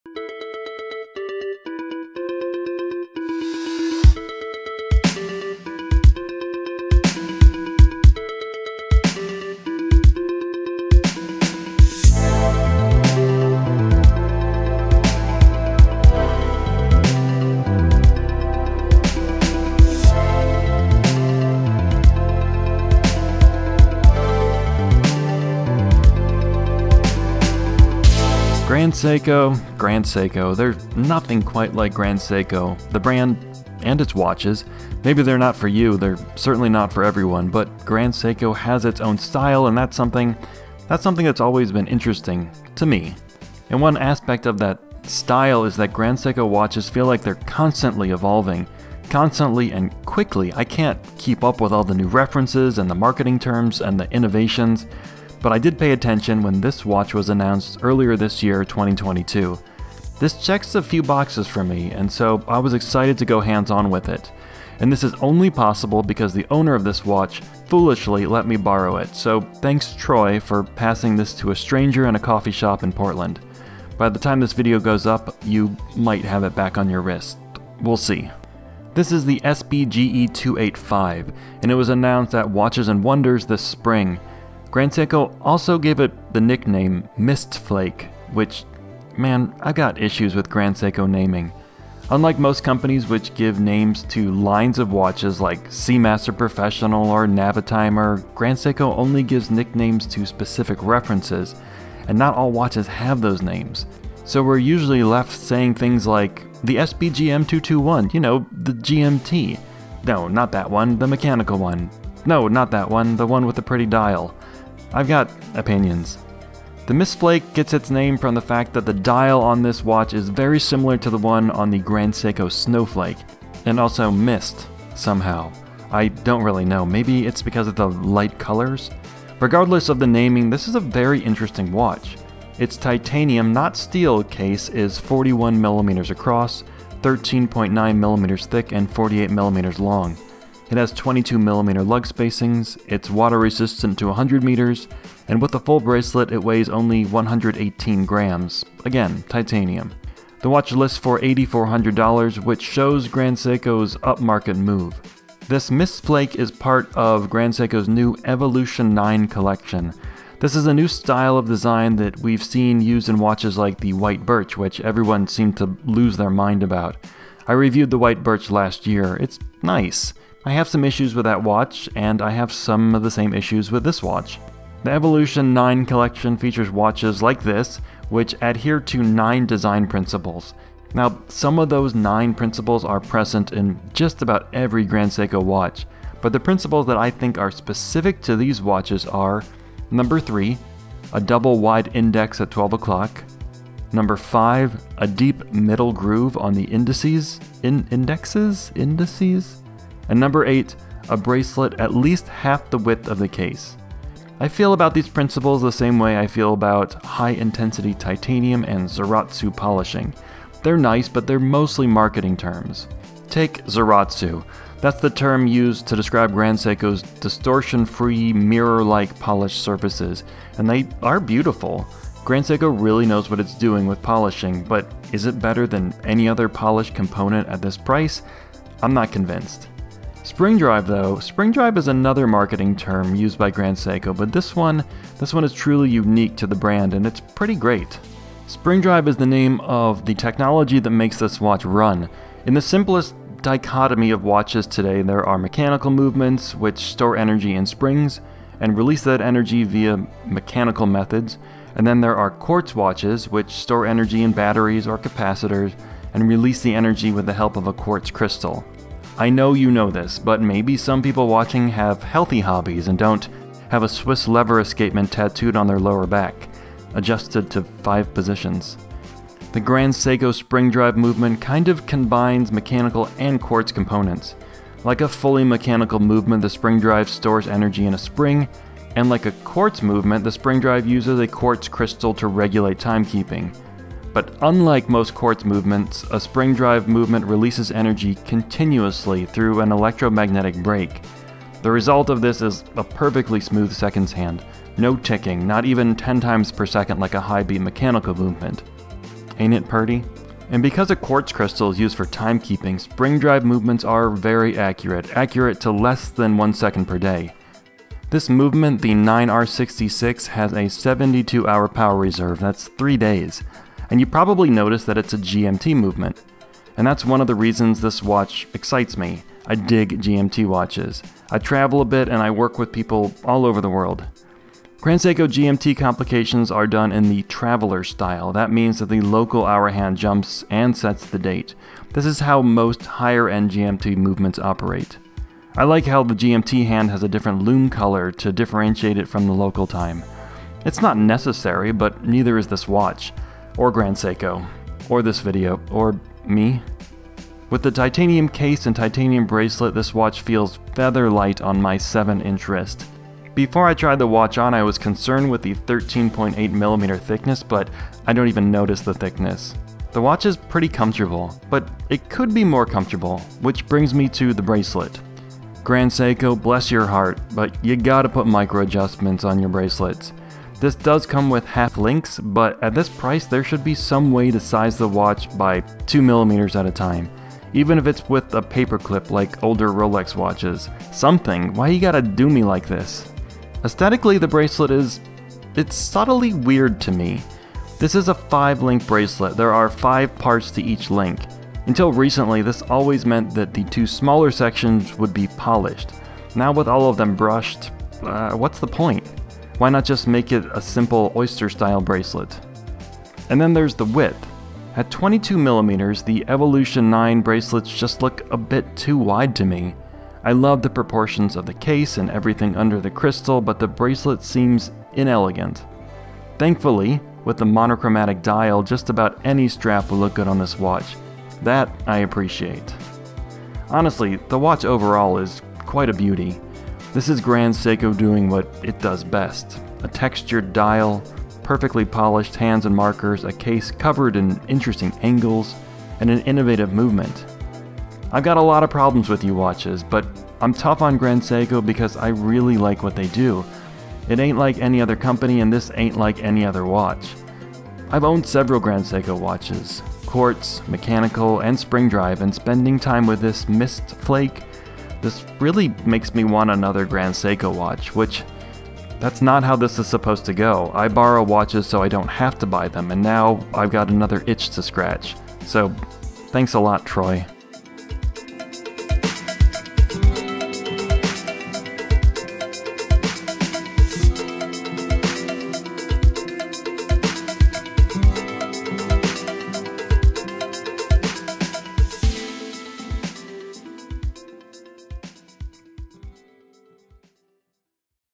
Grand Seiko Mist Flake GMT SBGE285
A look at the 2022 Mist Flake Spring Drive watch from Japanese manufacturer Grand Seiko